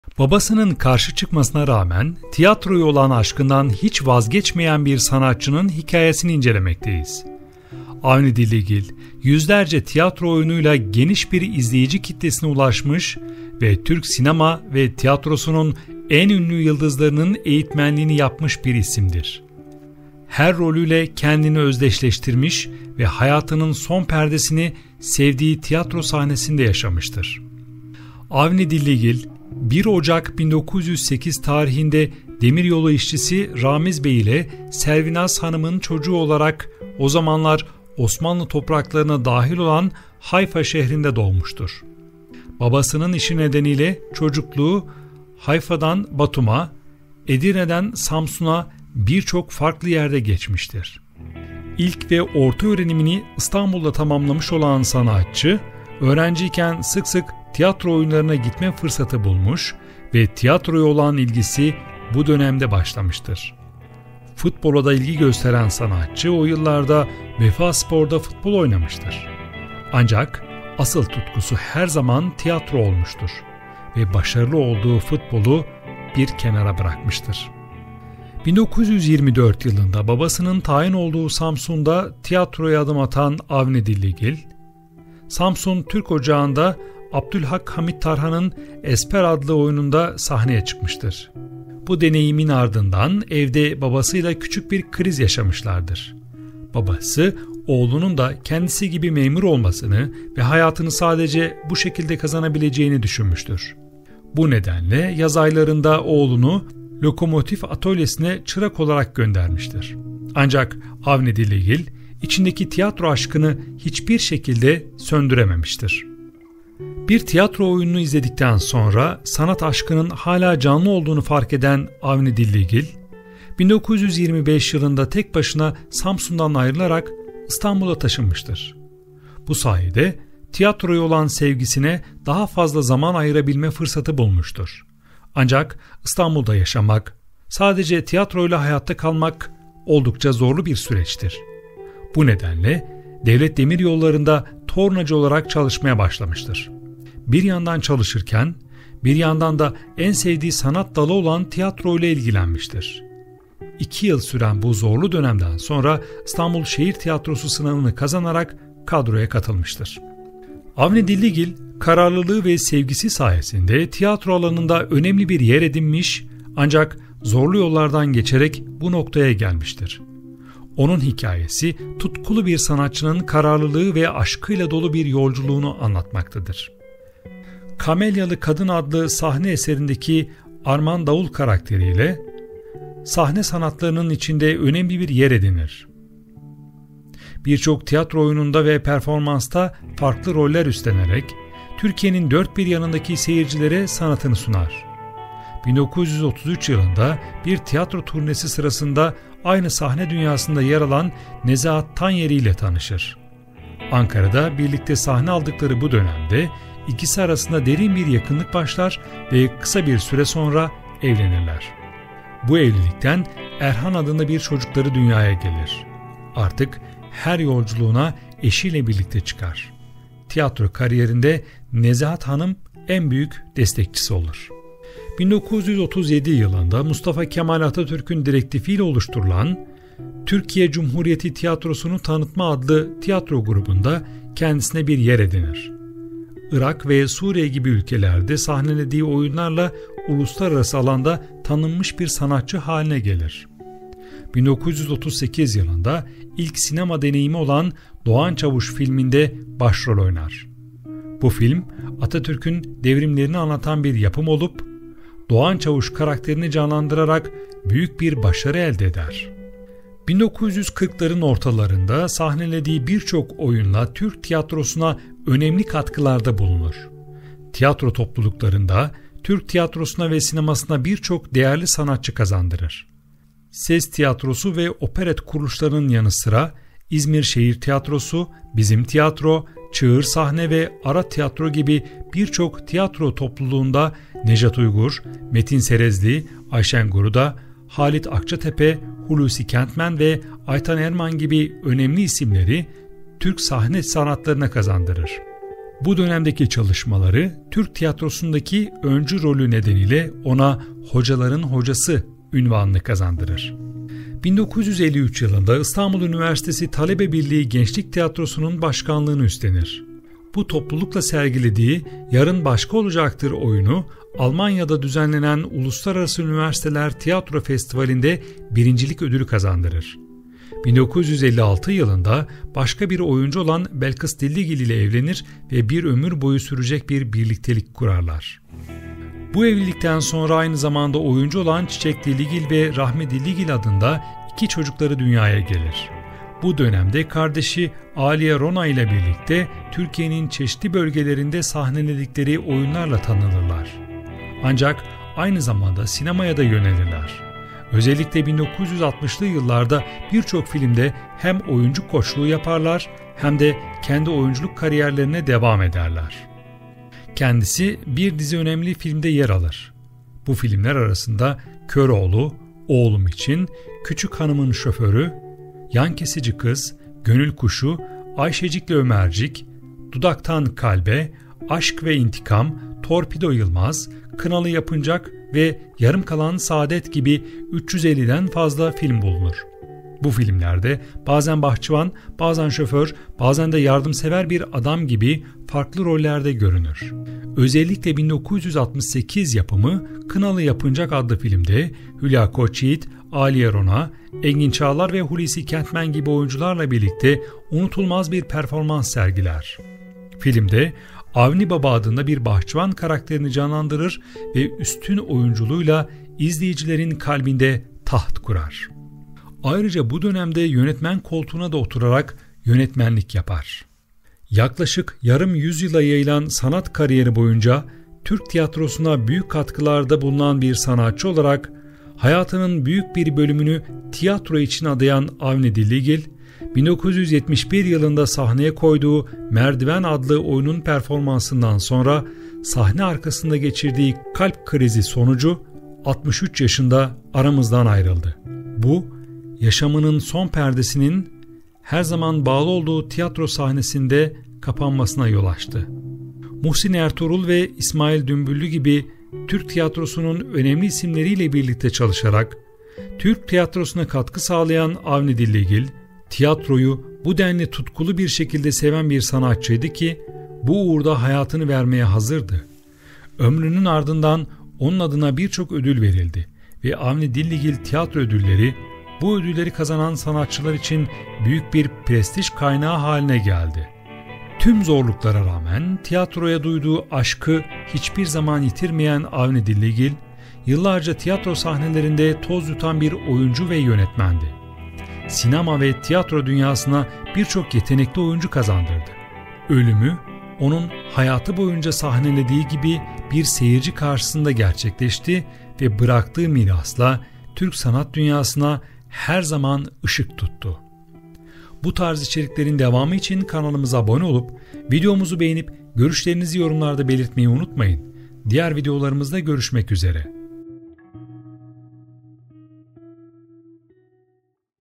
Kendisi Gibi Oyuncu Olan Kardeşi Kim? Avni Dilligil Kimdir?
Avni Dilligil, 1 Ocak 1908'de Osmanlı İmparatorluğu'na bağlı Hayfa'da doğdu ve 21 Mayıs 1971'de İstanbul'da hayata veda etti. Türk tiyatro ve sinema alanında önemli bir figür olarak kabul edilen Dilligil, oyuncu, yönetmen ve senarist olarak birçok eser vermiştir.
Eğitimini Edirne Lisesi ve İstanbul Lisesi’nde tamamladıktan sonra, 1927 yılında Şehir Tiyatroları’nda "Hamlet" adlı oyunda sahne alarak sanat kariyerine adım atmıştır. Uzun yıllar boyunca Şehir Tiyatroları'nda görev almış ve çeşitli tiyatro topluluklarında yer almıştır. Bunun yanı sıra, 350'den fazla filmde rol almış ve dört senaryo kaleme almıştır.
Dilligil, tiyatro dünyasında birçok yetenekli sanatçının yetişmesine yardımcı olmuş ve "Tiyatroda demokrasi olmaz" sözüyle anılmıştır. 1971 yılında Kadıköy'deki bir tiyatro kulisinde kalp krizi geçirerek hayatını kaybetmiş ve Karacaahmet Mezarlığı’na defnedilmiştir.